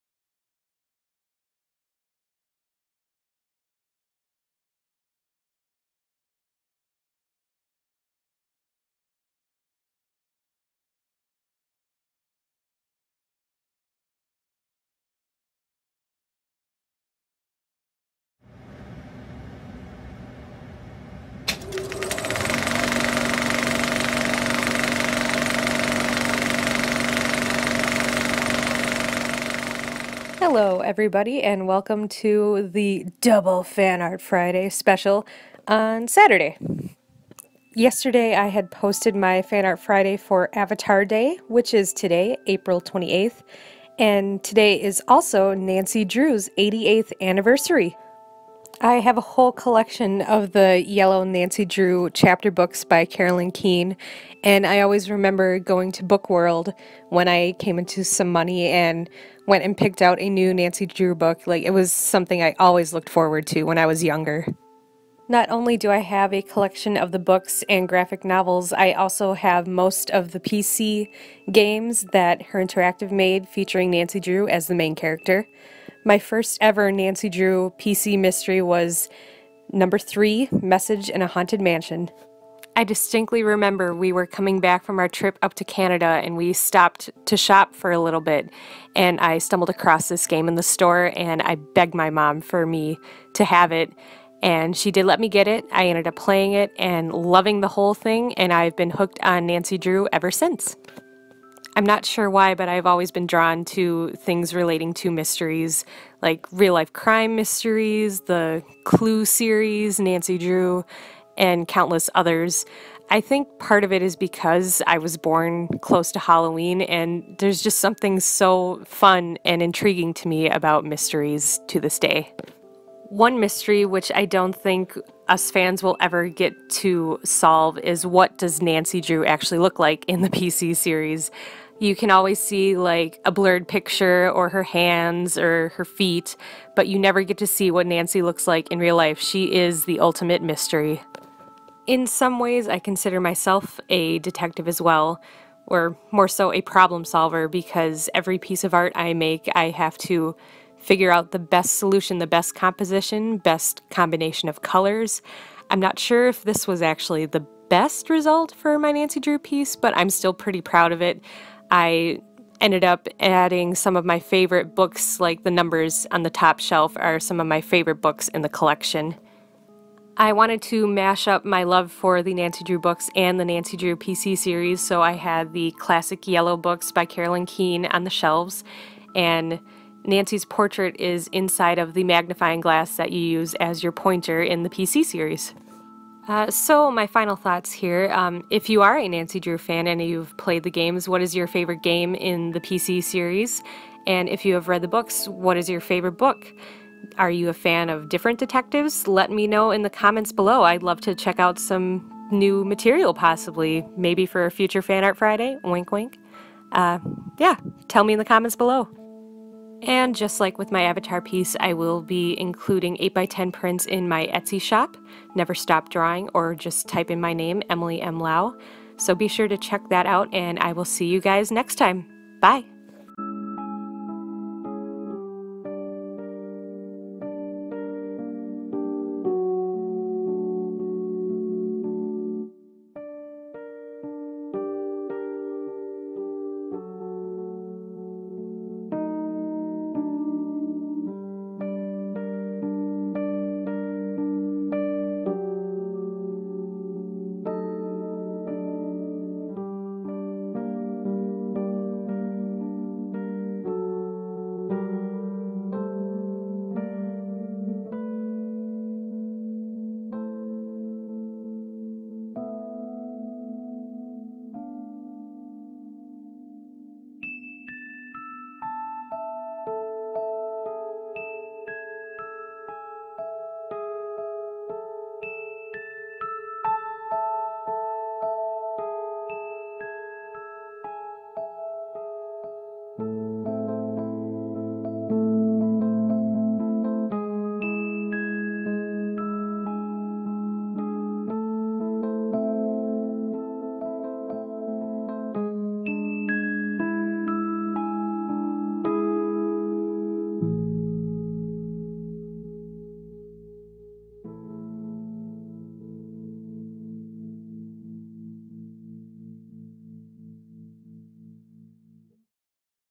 Nancy Drew's 88th Anniversary | Acrylic Painting
Bonus Fan Art Friday video!!! This one features my favorite girl detective, Miss Nancy Drew!

Check me out on the other social medias!

Etsy Shop